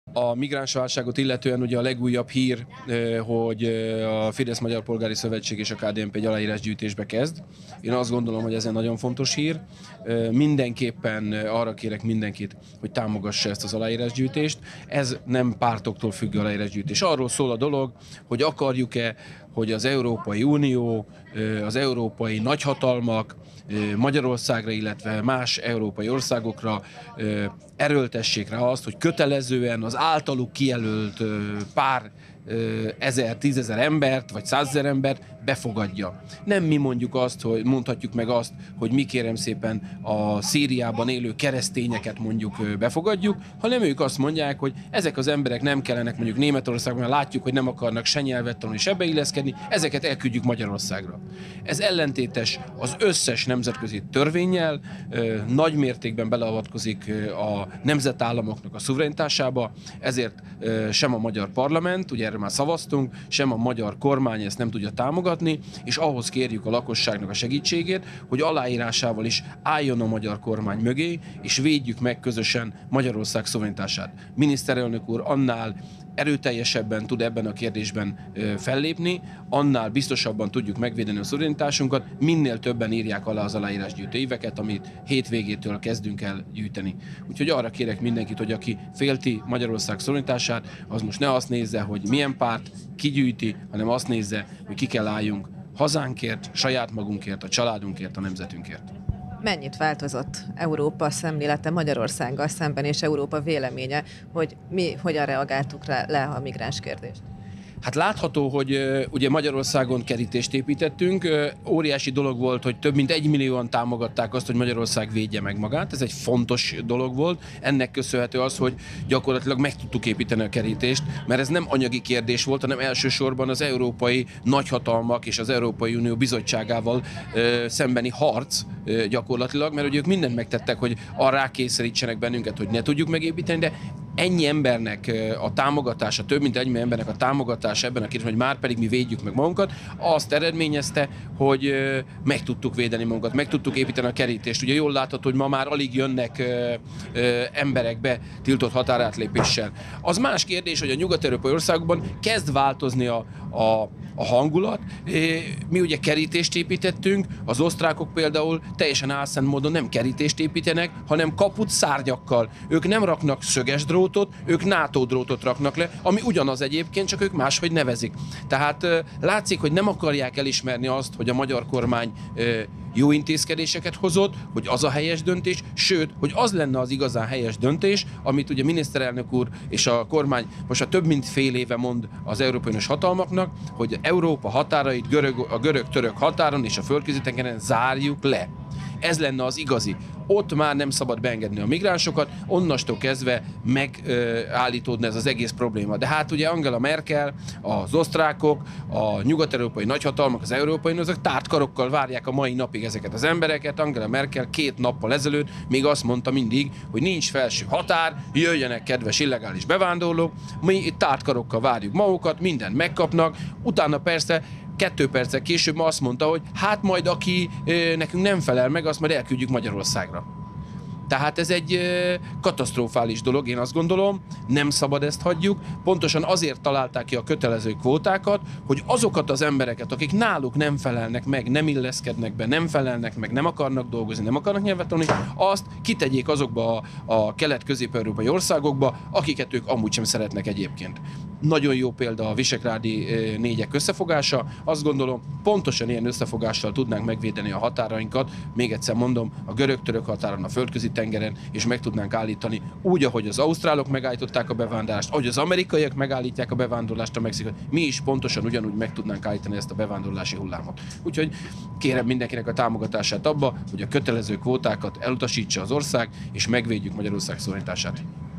2015 11 05 Boldog István a migránskvóta elleni aláírásgyűjtésről
A térségben az első!
Törökszentmiklós és Térsége Televíziója!